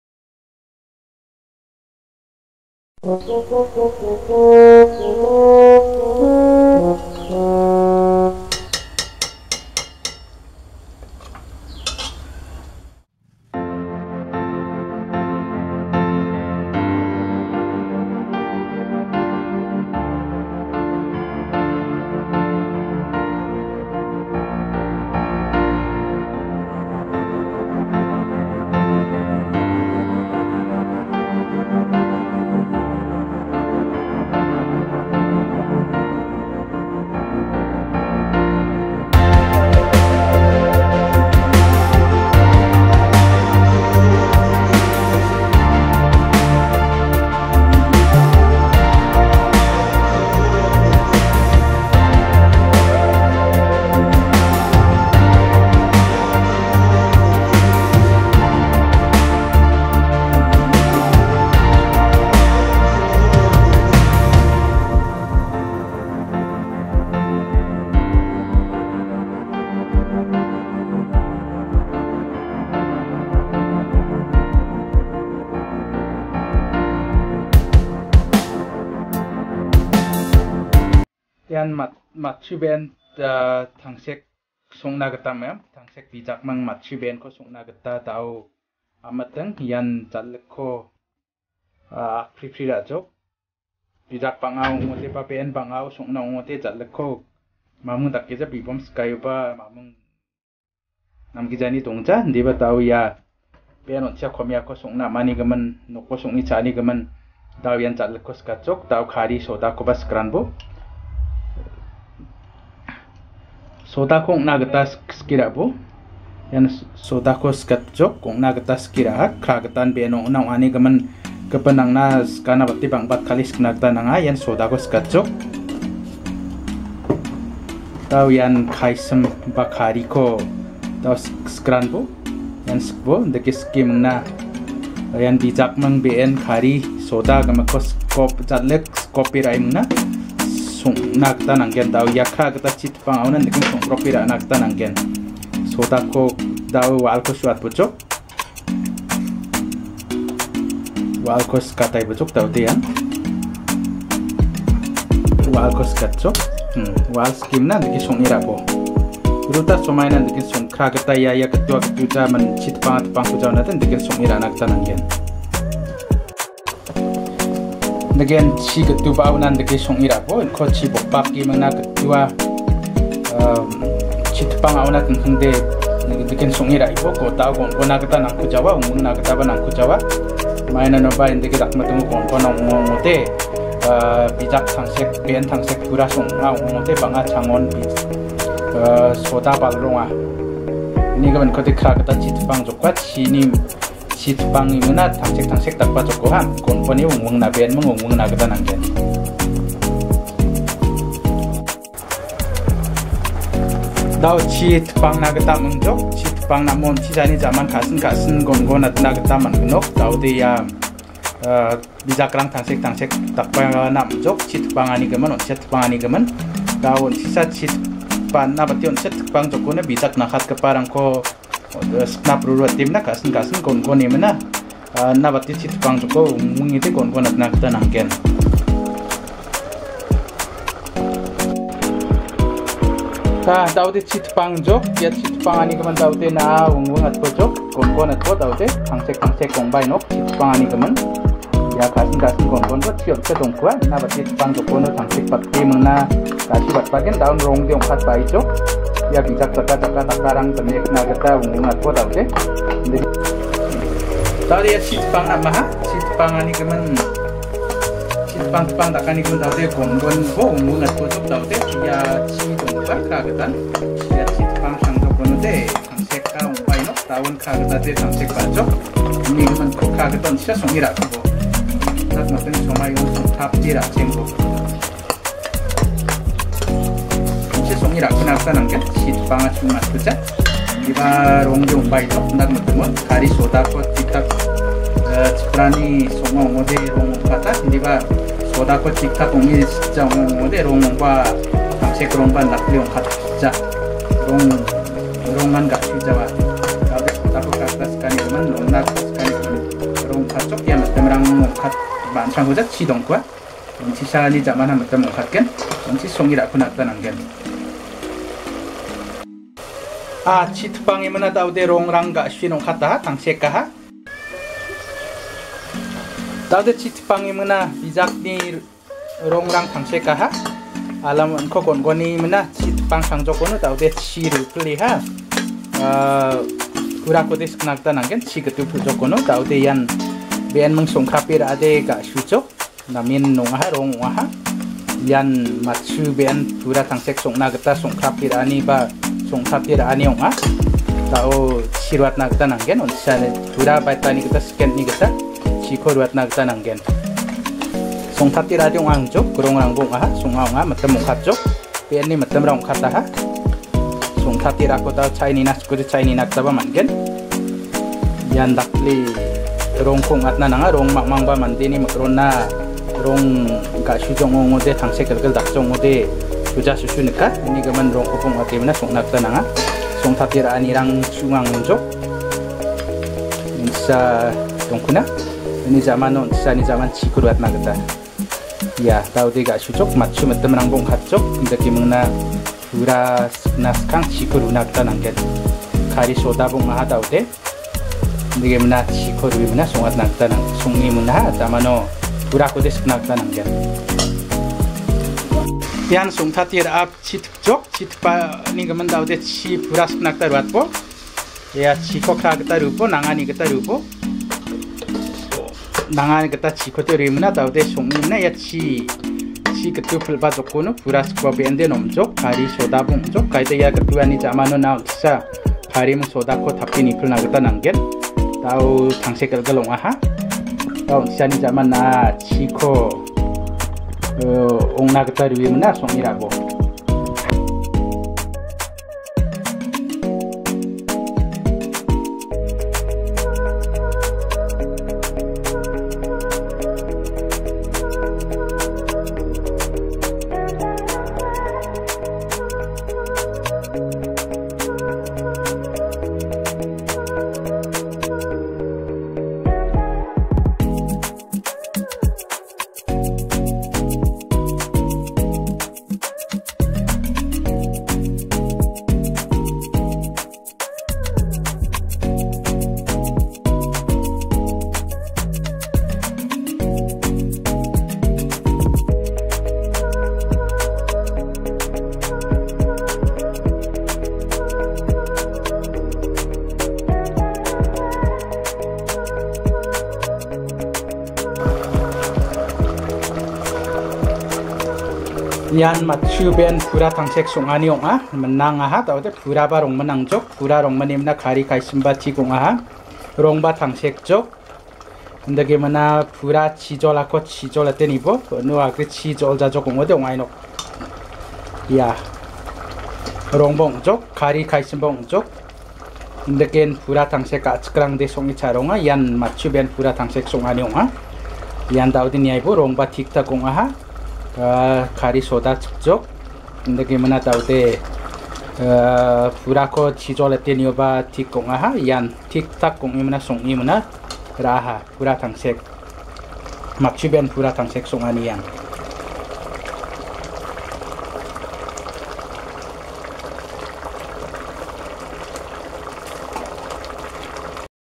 Traditional Garo Dish | Matchu pura tangsek // Beef with green leaves rice flour gravy
A·chikrangni song·e cha·ronggipa Matchu tangsek pura song·aniko song·e mesokatani.
Traditional Garo Dish beef with green leaves and rice flour gravy. Illustrate how to cook Beef with any locally available green leaves and rice flour. It is easy to cook, but one of the most popular and tastiest among Garo dishes.